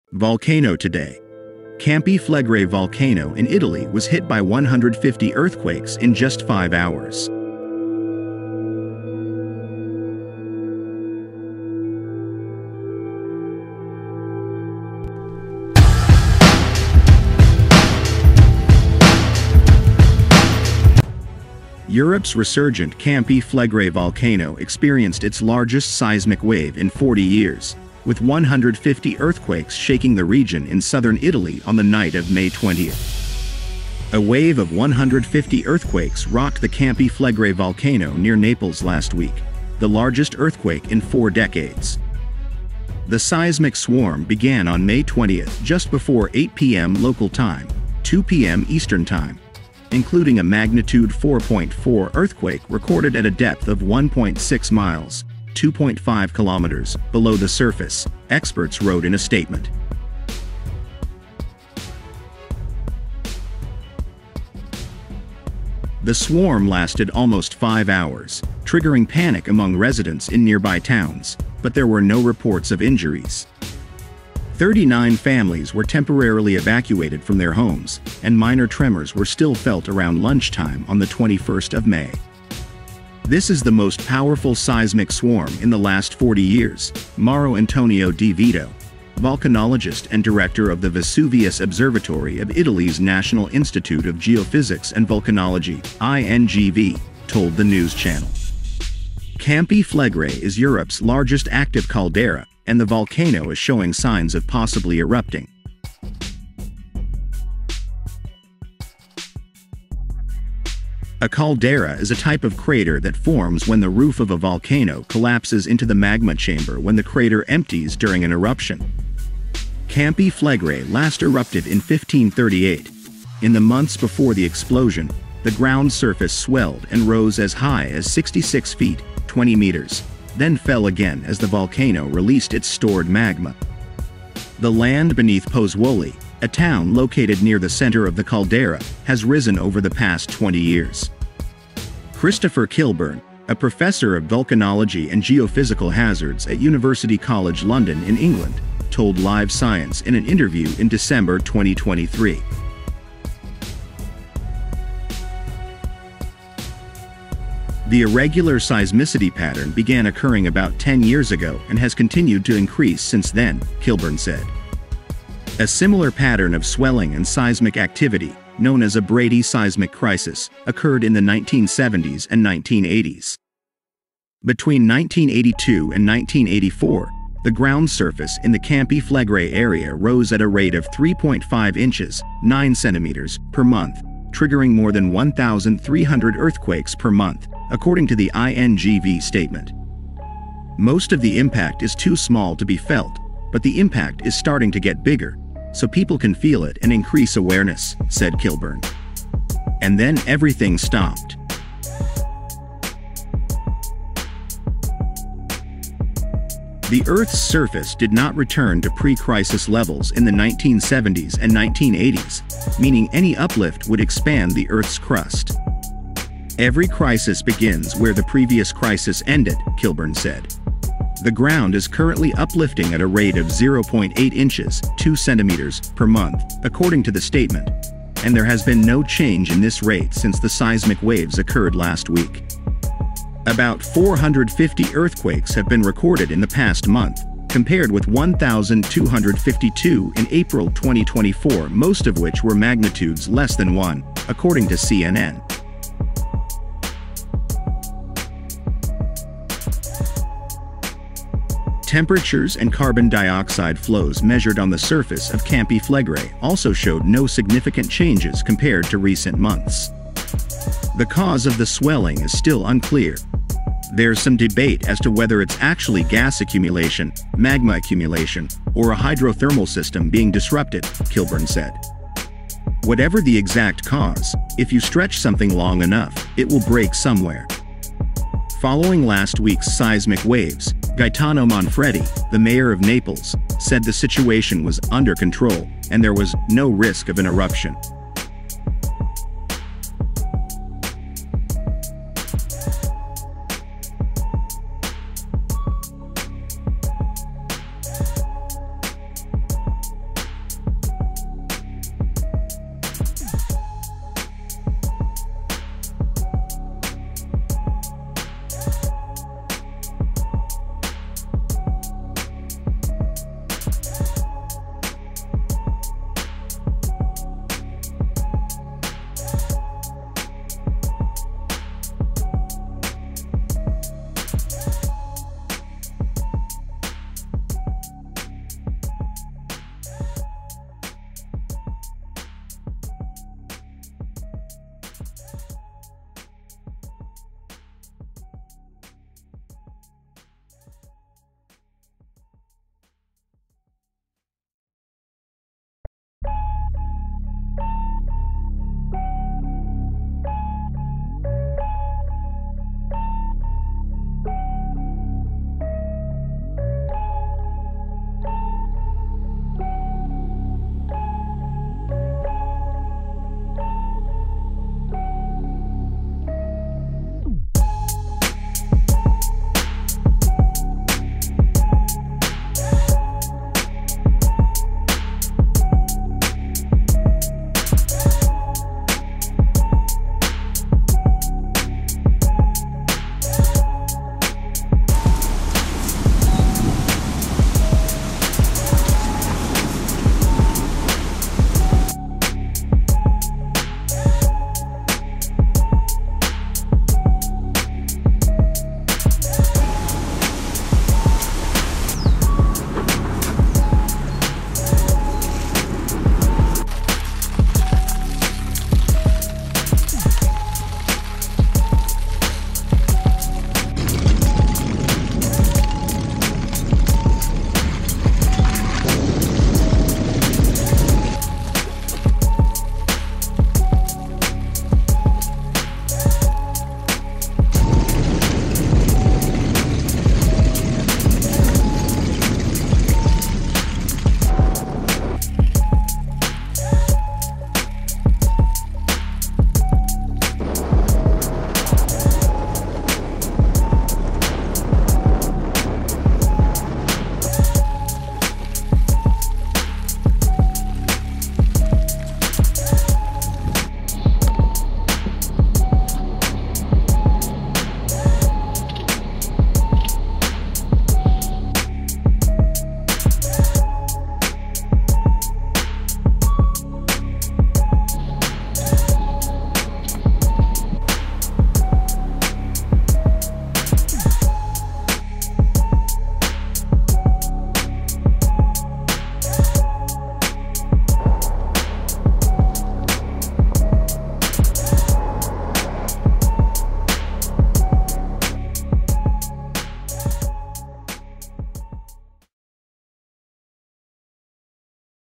Italy panic: 5 hours ago, When Campi Flegrei volcano was hit by 150 earthquakes,rumbling across land
Europe's resurgent Campi Flegrei volcano experienced its largest seismic wave in 40 years, with 150 earthquakes shaking the region in southern Italy on the night of May 20. A wave of 150 earthquakes rocked the Campi Flegrei volcano near Naples last week – the largest earthquake in four decades. The seismic swarm began on May 20 just before 8 p.m. local time (2 p.m. ET), including a magnitude 4.4 earthquake recorded at a depth of 1.6 miles (2.5 kilometers) below the surface, experts wrote in a statement .